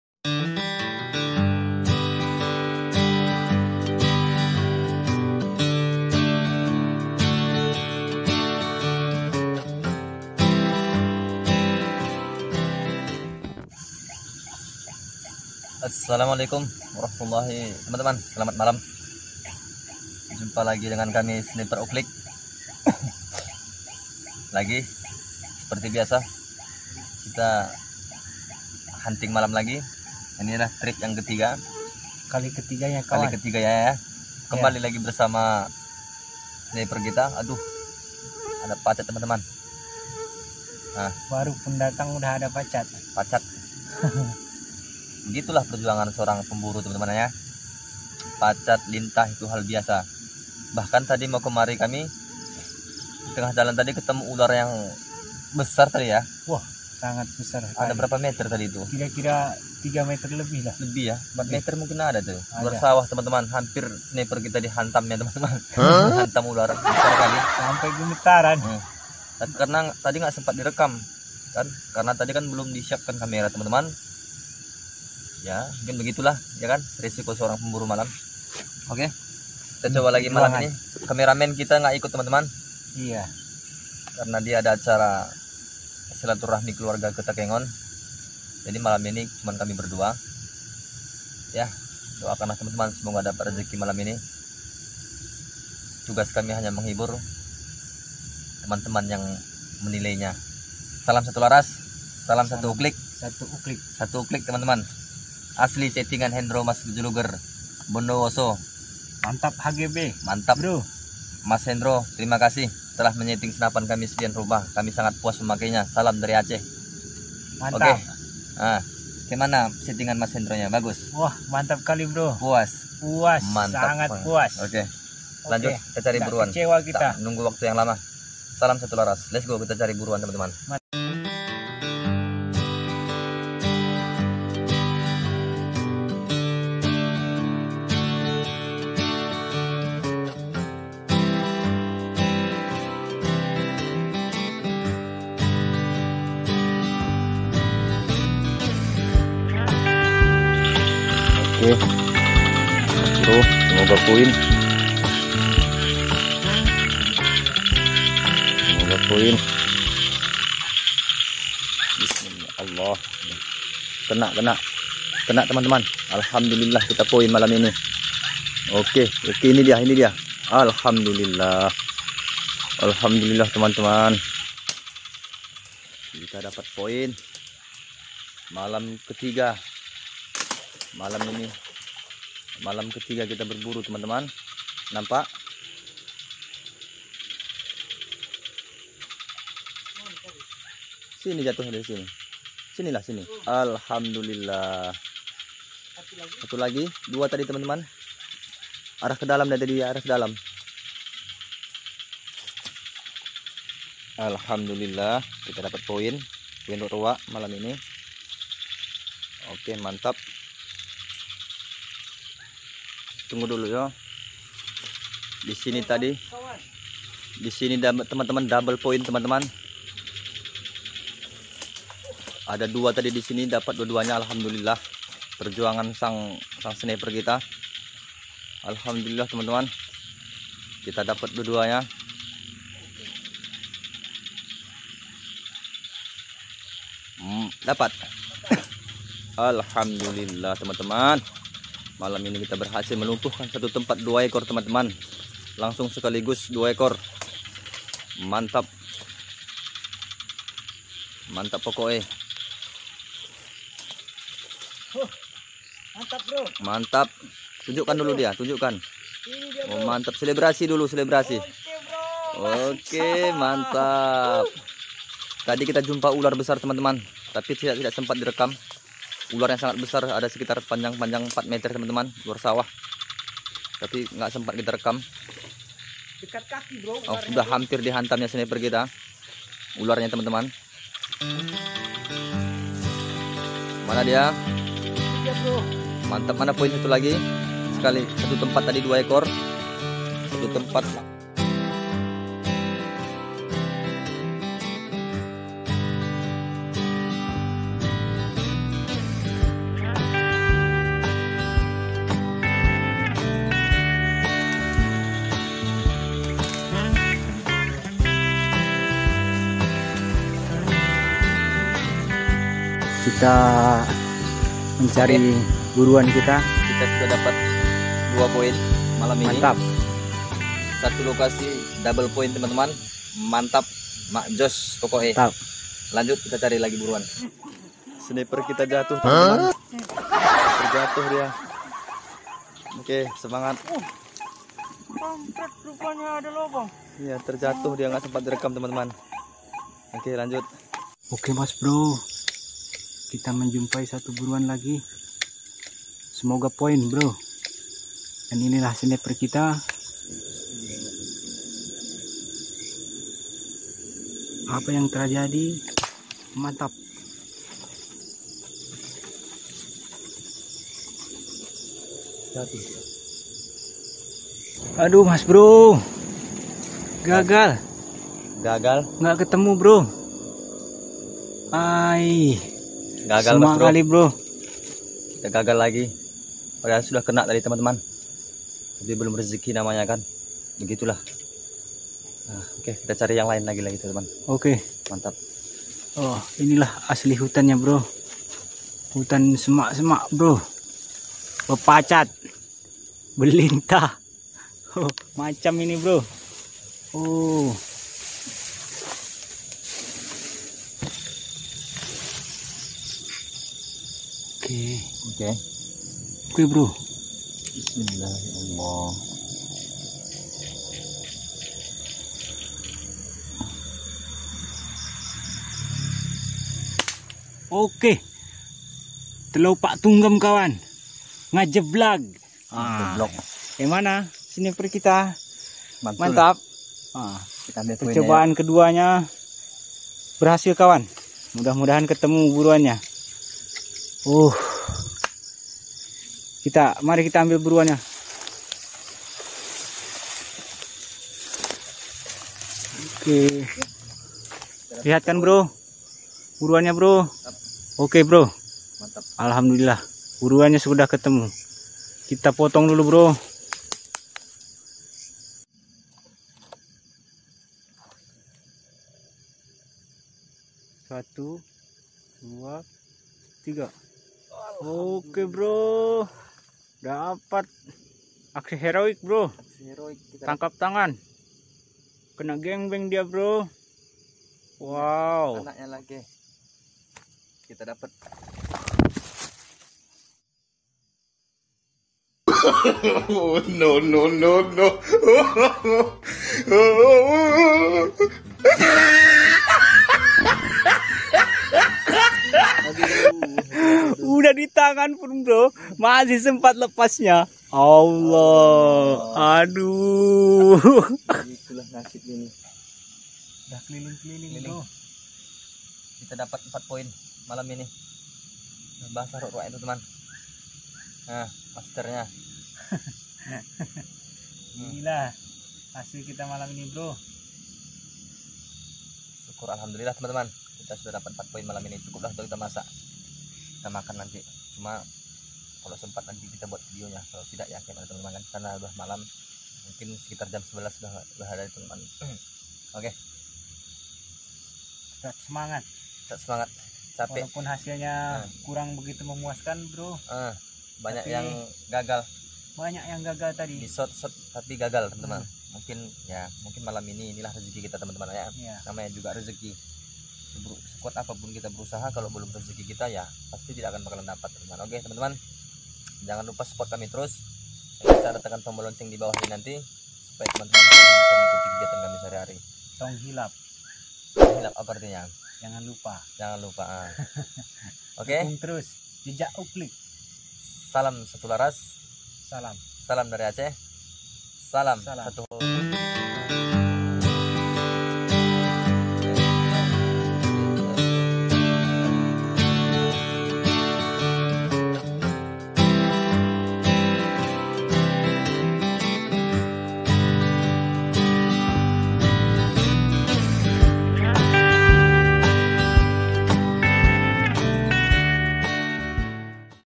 Berburu Ruak Ruak di malam hari dengan senapan uklik (part 3)
berburu Ruak Ruak dimalam hari
sangat seru dan menyenangkan